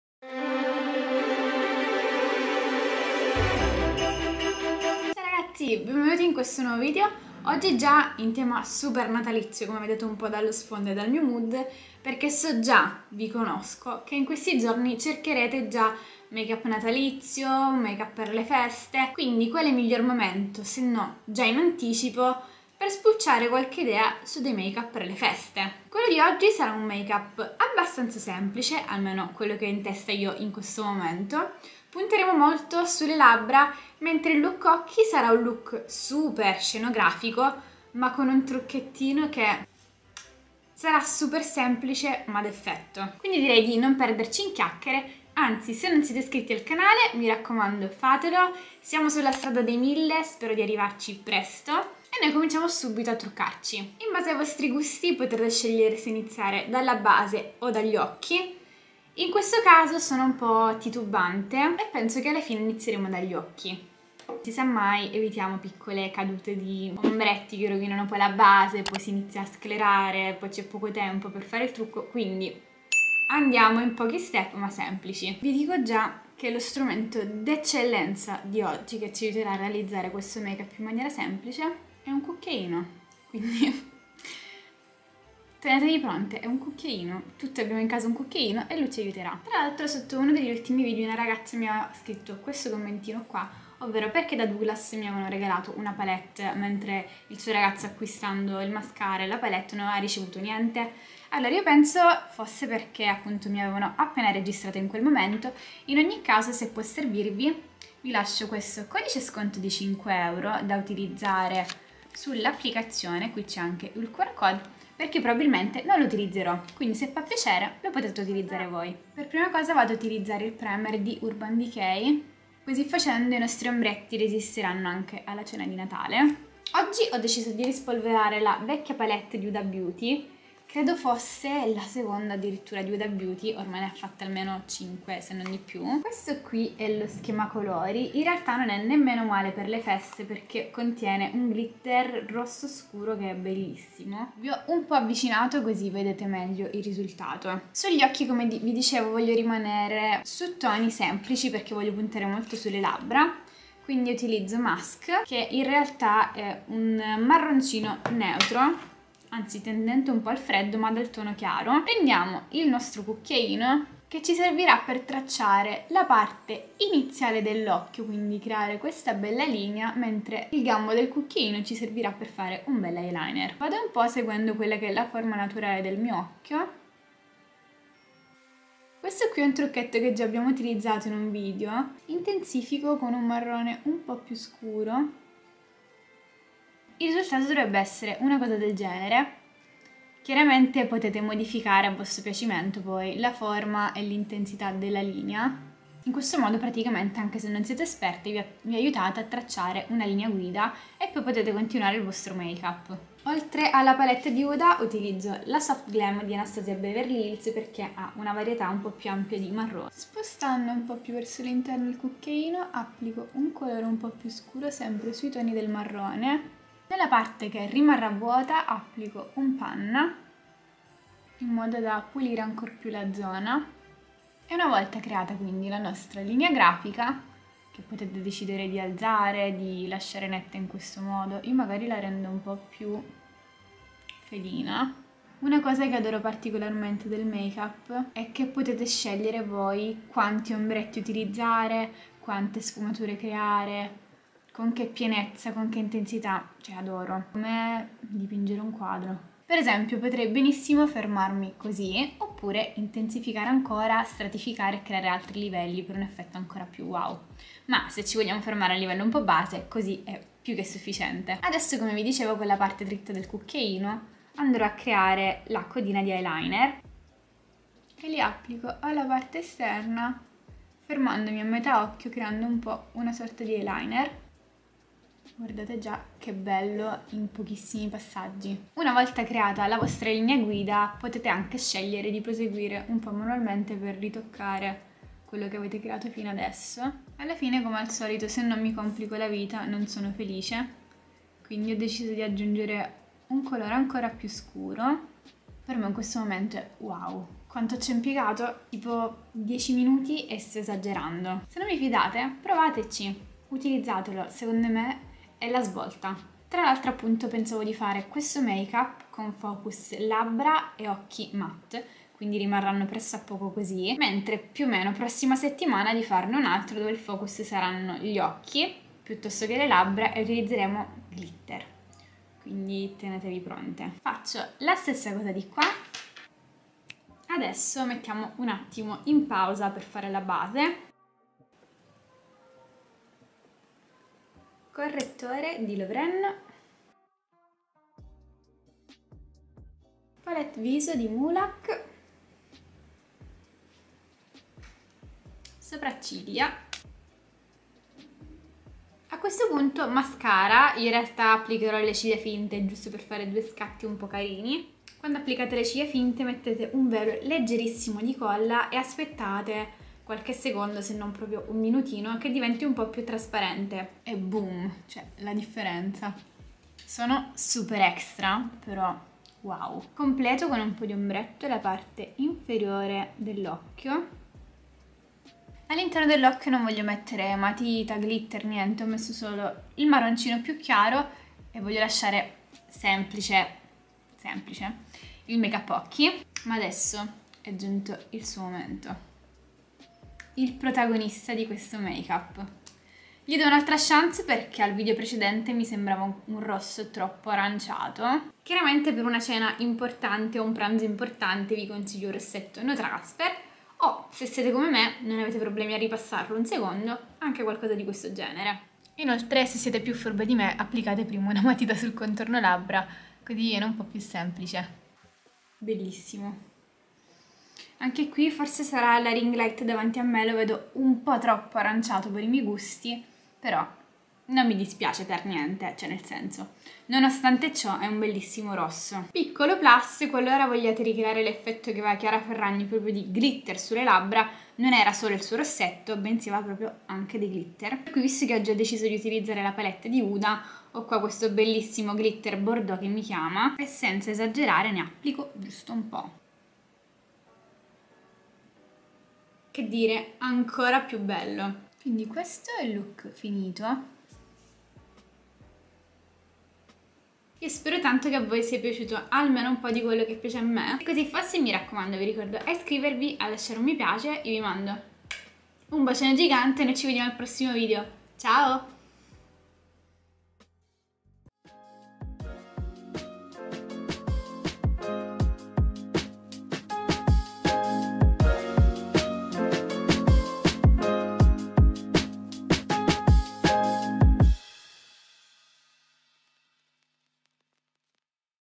Christmas makeup tutorial, per principianti | TheJess
Ciao ragazzi!

Oggi makeup Natalizio, focalizzato sulle labbra!
Abbiamo ricreato delle labbra glitter rosse, immancabili, ed un trucco occhi all'apparenza complesso, che in realtà si ricrea facilmente con...un cucchiaino...
Hai capito bene, UN CUCCHIAINO!!

Ebbene sì, è stato più semplice del previsto, tu conoscevi già questo trucchetto?

Grazie per tutto,

Jess